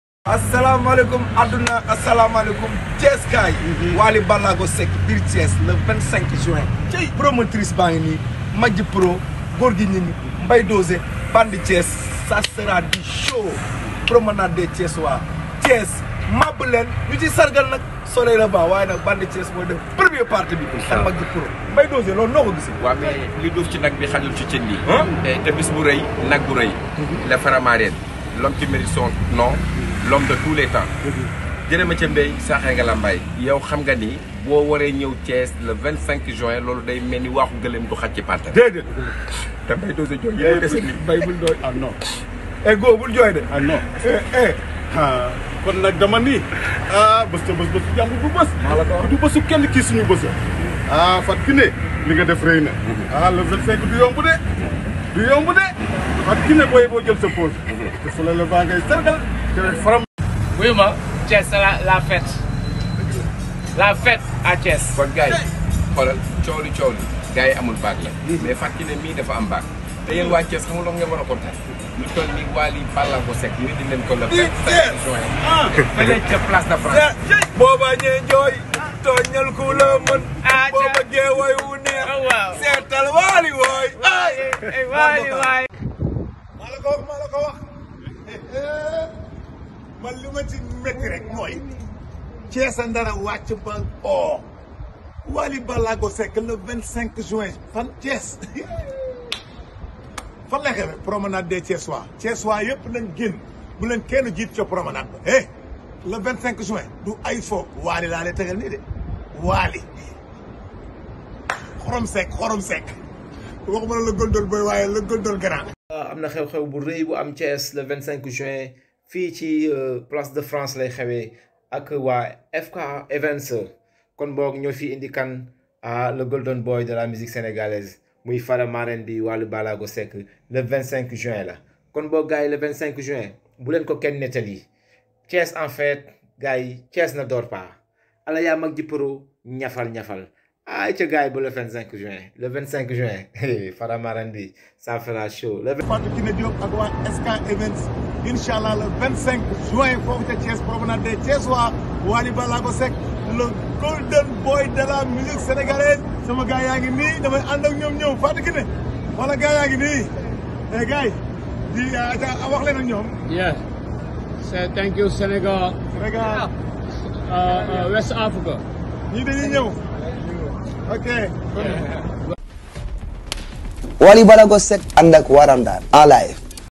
Thiés en fête" rendez-vous le 25/ juin avec Wally B. Seck promonade des Thiésois
N'oubilier pas de vs abonnè sur votre chaine YouTube Wally_info_urgent retrouver tout l'actualitè de Wally B. Seck an temp reel icii